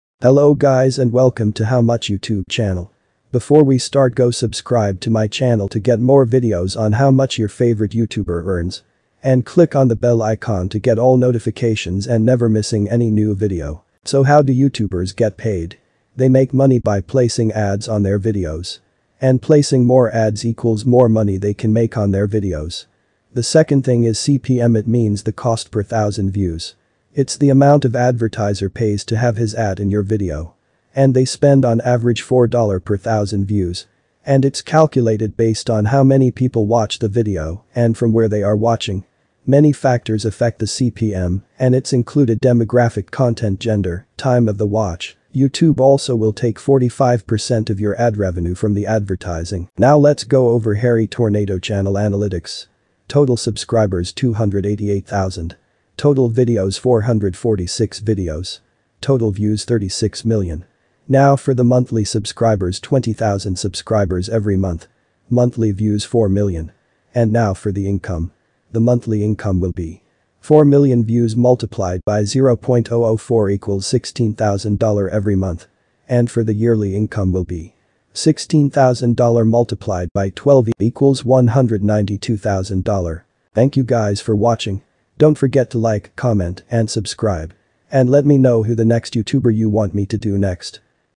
How Much Does Hairy Tornado Make on Youtube
In this video, I will show you the estimate of how much money does Hairy Tornado makes on YouTube.